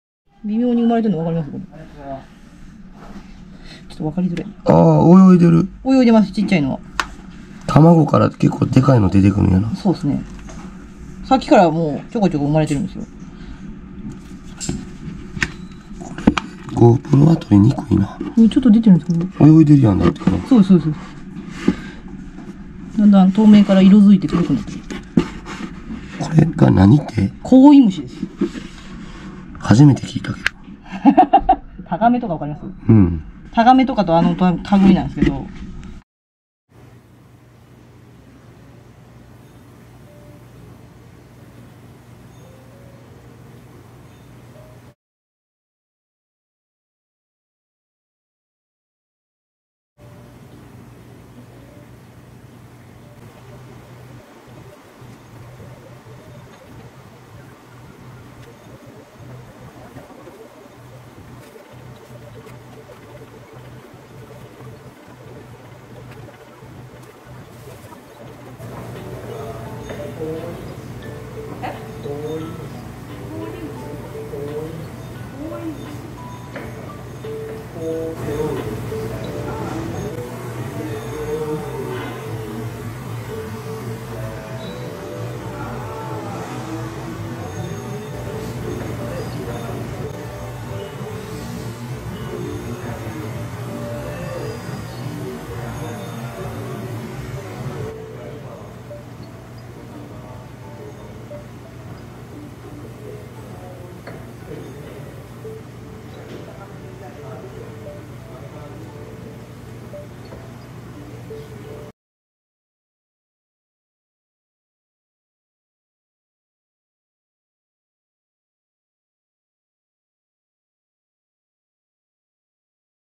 【お魚関連】みんなも覚えて！ 館長も初めて見た虫！ （伊勢シーパラダイス）
恥ずかしながら。。。海獣類以外の生きものについてとっても疎い館長。
その分、知っているスタッフには根掘り葉掘り鋭く質問引かれますが、初めて聞いた、初めて見た水生昆虫であるコオイムシの孵化の様子です。
タガメやミズカマキリたちと共に、だいぶ数を減らしている昆虫です。
寿命はそれほど長くはないと思いますが、今現在まる見えバックヤードで飼育しています。

■各リンク
🔵伊勢シーパラダイスチャンネル